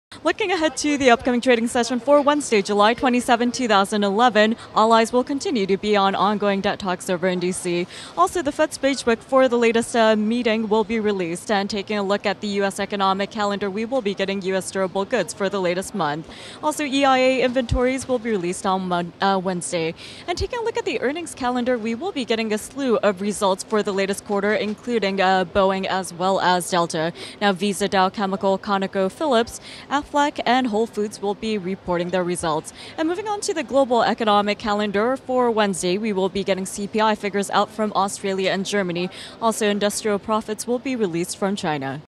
The Day Ahead: Earnings - Aetna, Aflac, Boeing, ConocoPhillips, Delta, Dow Chemical, Symantec, Visa,
07/26/11 The stories, data, and stocks that may have the greatest impact during the next trading session.